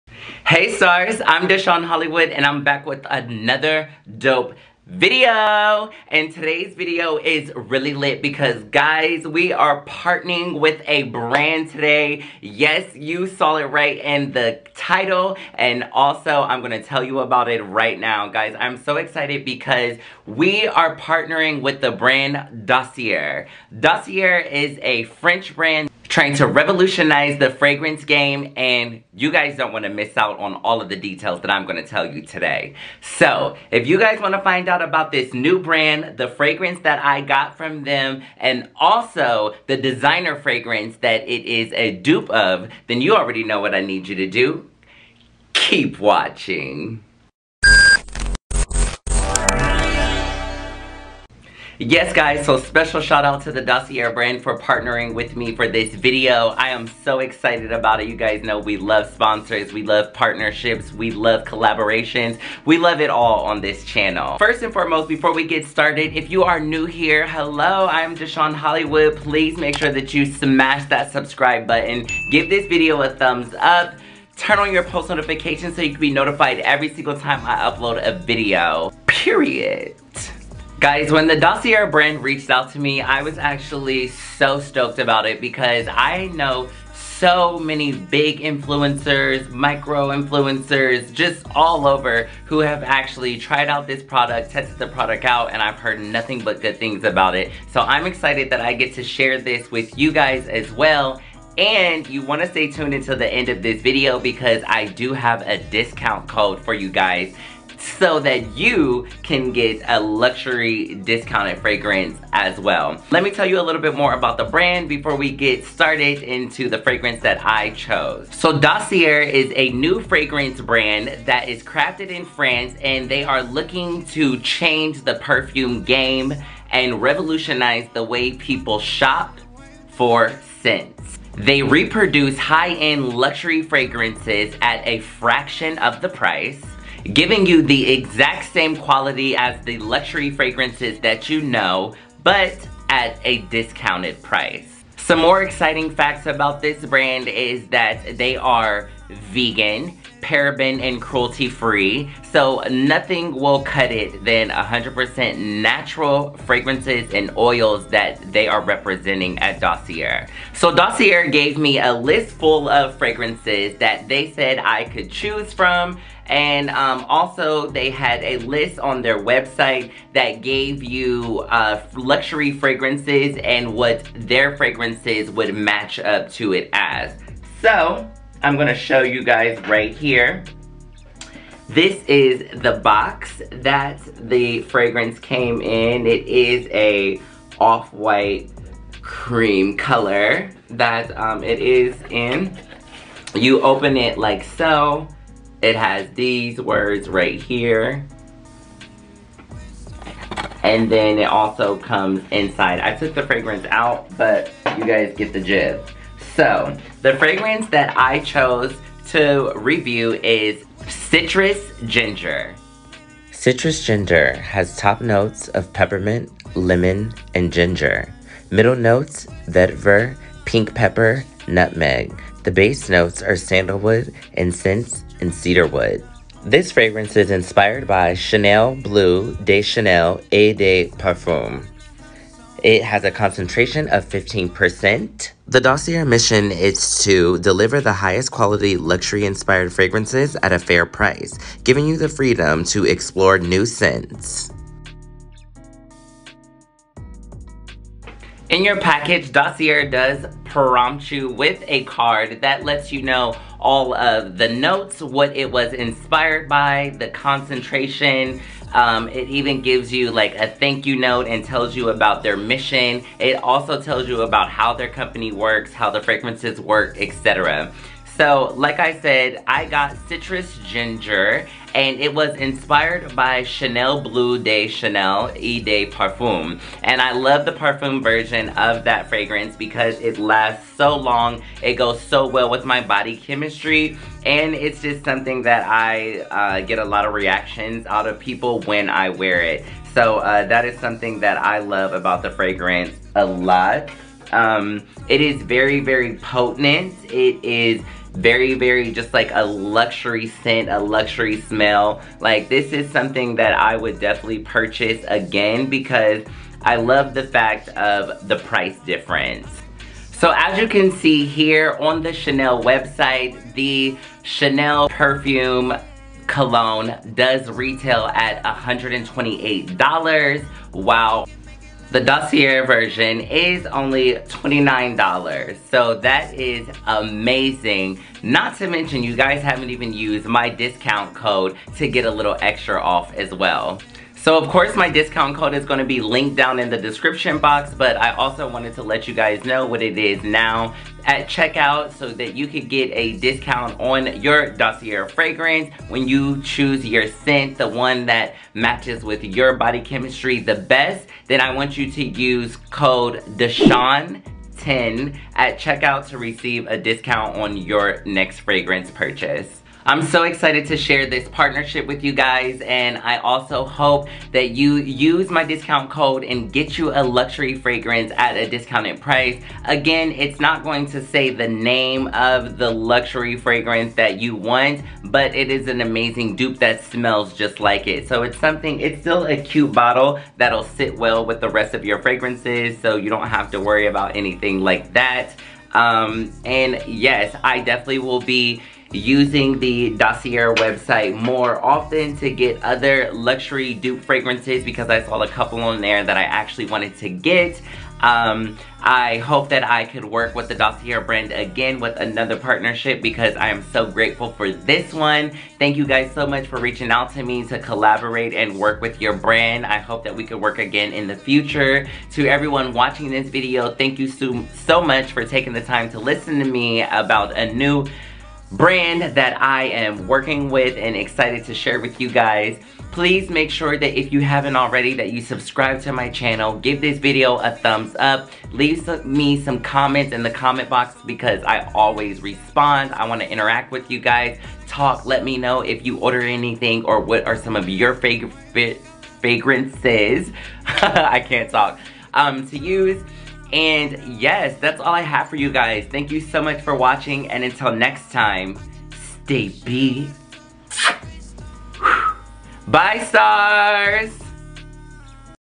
GET LUXURY FRAGRANCE SCENTS ON A BUDGET | DOSSIER | CITRUS GINGER (INSPIRED BY CHANEL BLEU)‼️
HEY STARZ✨

Check out my new video of my partnering with the Dossier brand so I can share the word on how you can have the scent of your favorite luxury fragrance at a discounted price. Men’s, Women’s, Unisex you name it they have it.

Silver but Gold [yea...I’m black] Apparel

If you would like to donate anything to the DeShawn Hollywood Brand